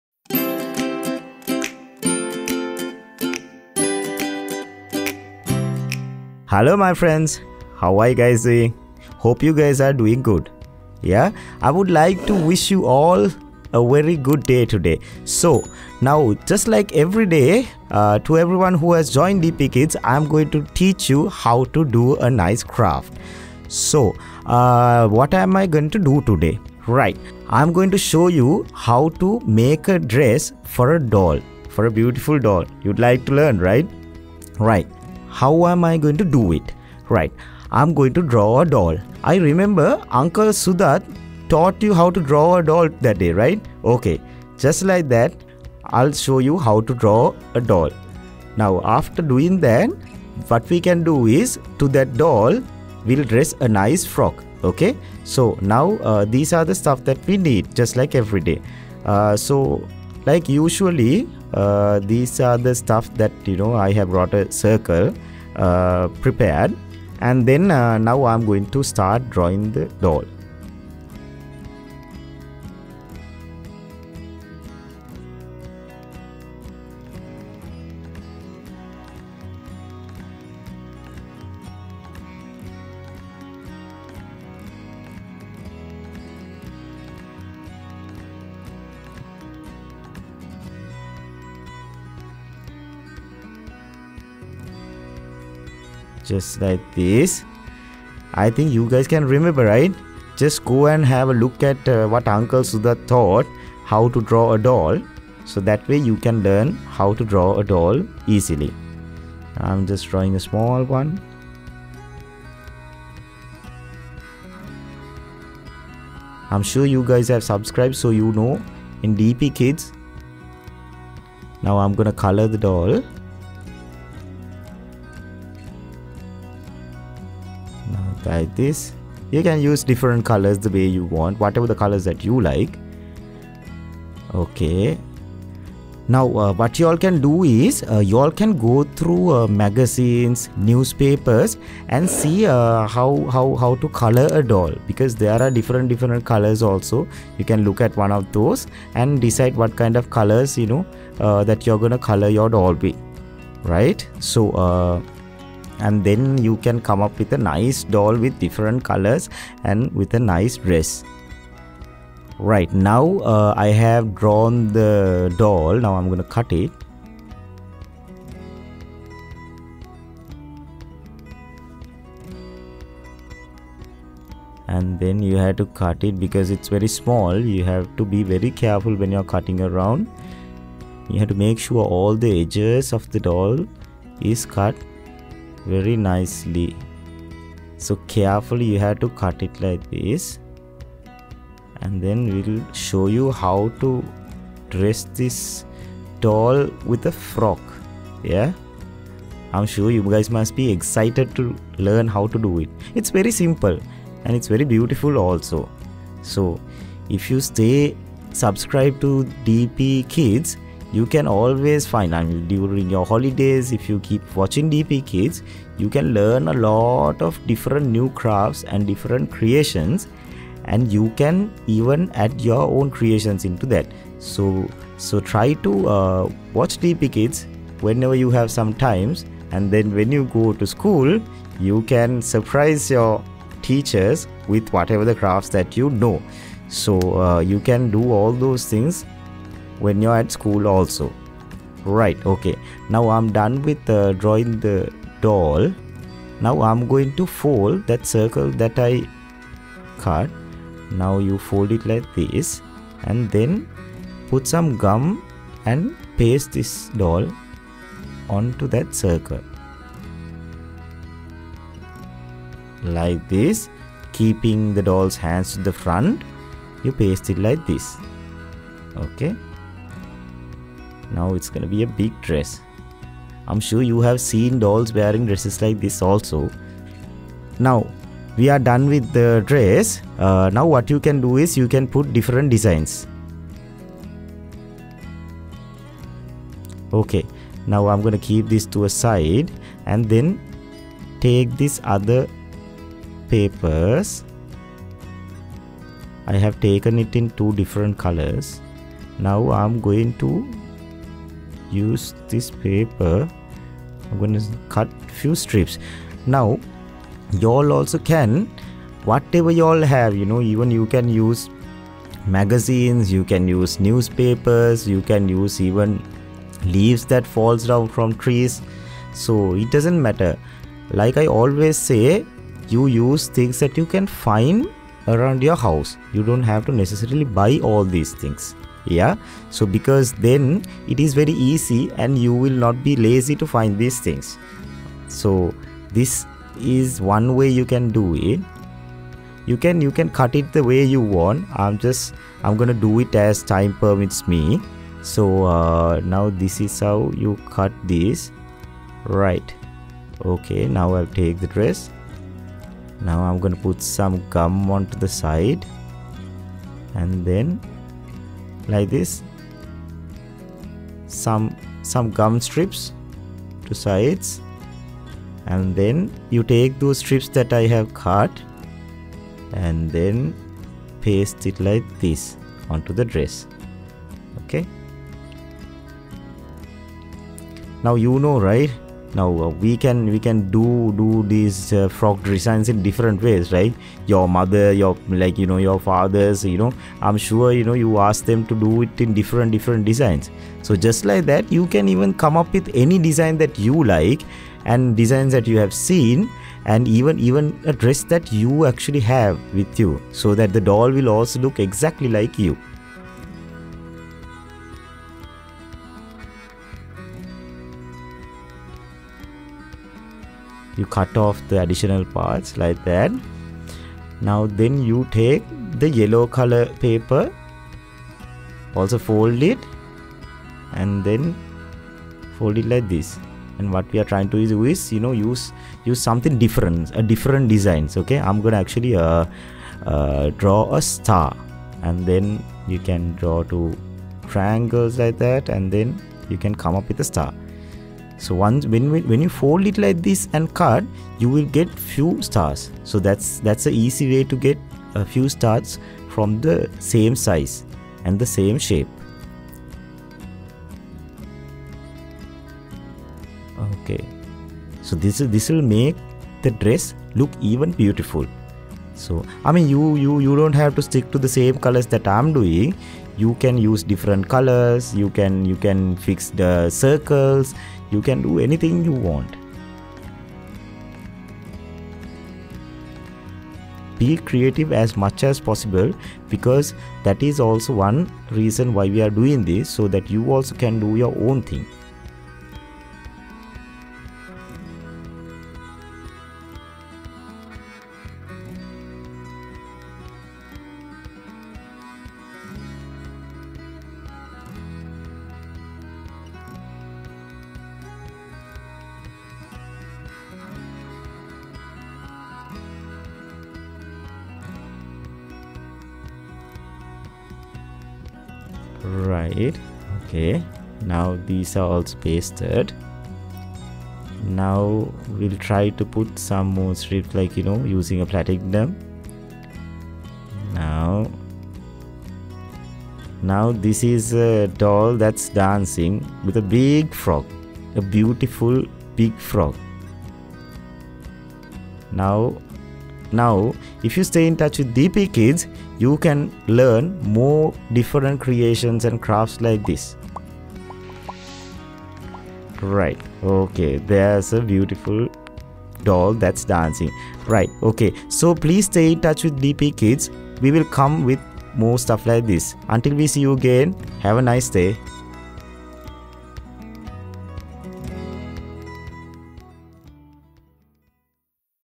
Let's make a paper frock for doll - DP KIDS CRAFTS (Video 151)
▶️ In this video, we will see how to make  a   paper frock for doll
TIMESTAMPS:
Introduction - 0:00
Let's make a  paper frock for doll  -  DP KIDS CRAFTS (Video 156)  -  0:40